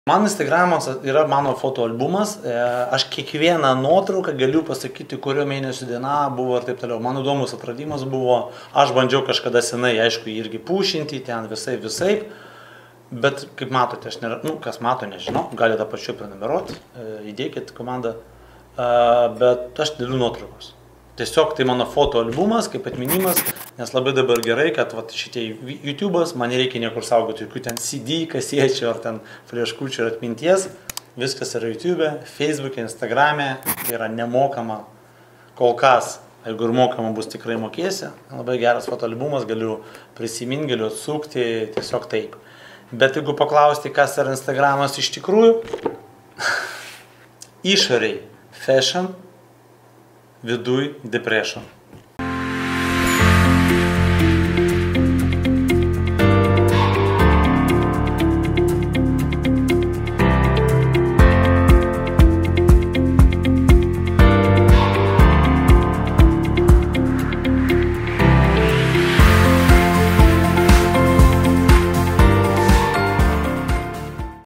Instagram Fashion vs Depression. "Feikas" ir "Realybė". Jevgenijus Černyš
Instagram'as jau lenkia Facebook savo vartotojų aktyvumu ir registracijų skaičiumi. Visas gyvenimas vyksta čia. Nuotraukos, įspūdžiai... Tačiau kas iš jų yra tikra, o kas yra klastotė "feikas"?

Patiko? Pasidalink!